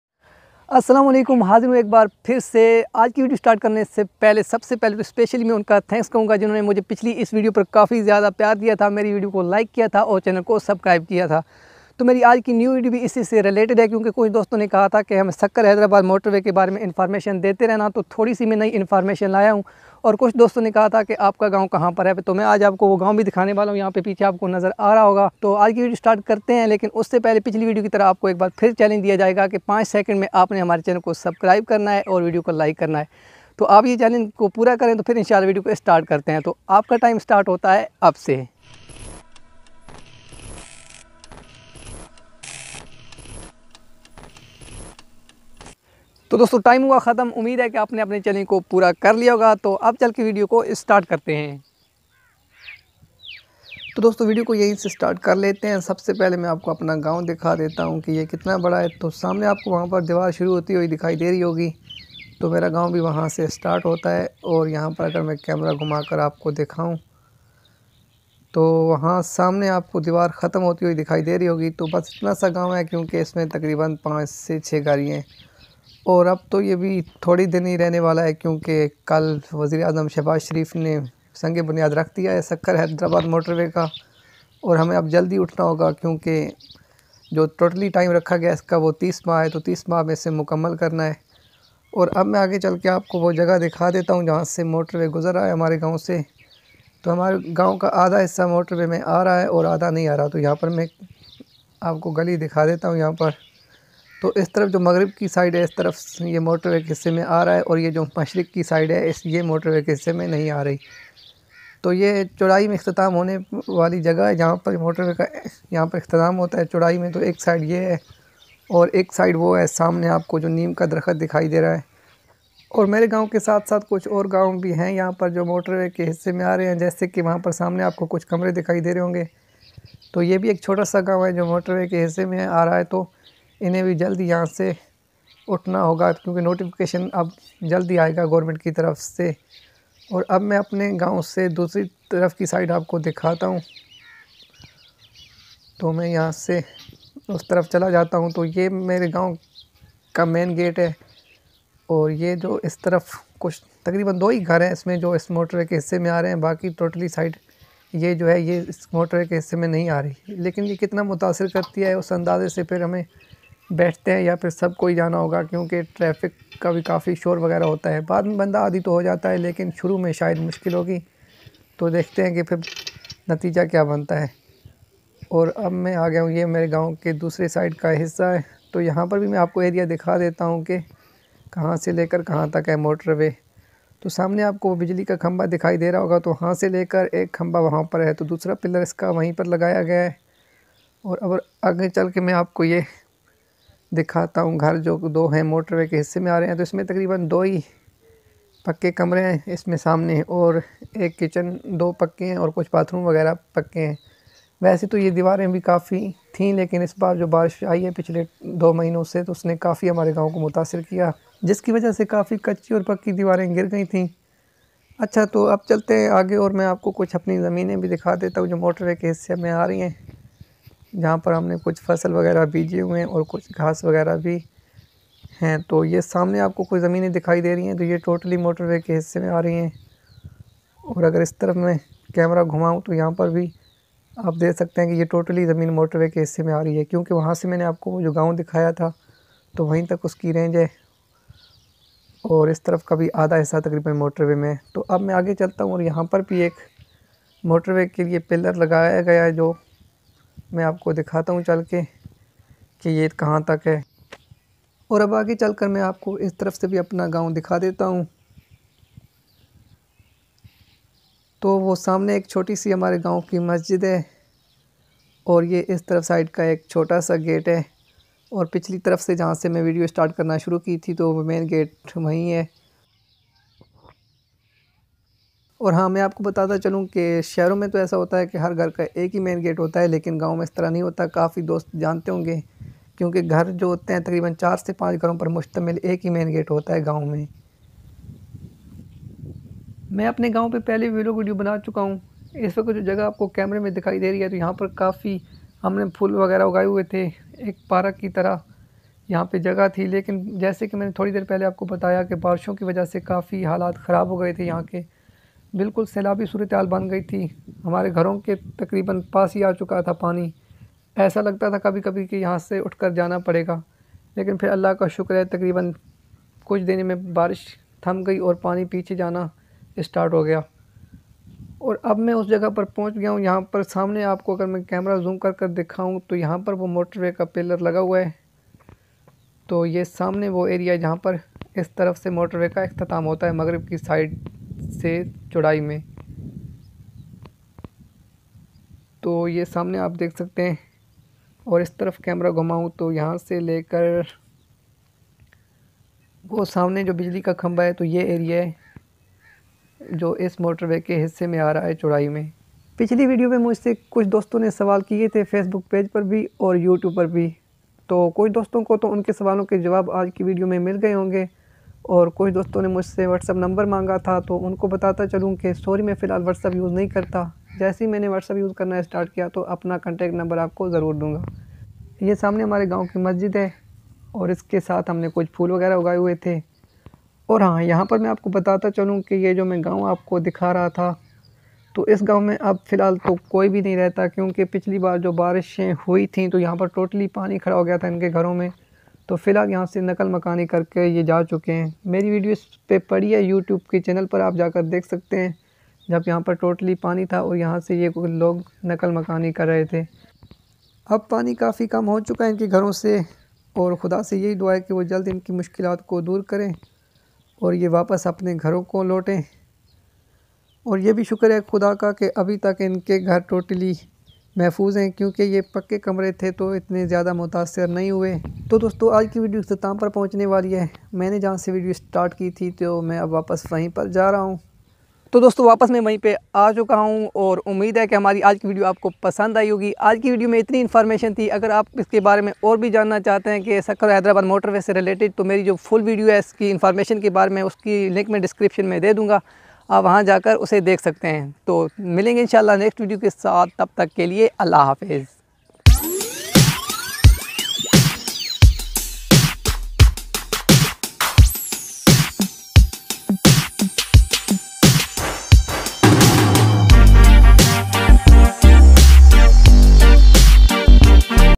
Sukkur Hyderabad Motorway Construction Started Latest Update
Sukkur Se Hyderabad Tak Jane Wala Motorway Par Kam Shoro Karne Ka Aelan Kar Diya Gaya Ye Kab Tak Moqamal Ho Jae Ga Janiye Is Video Main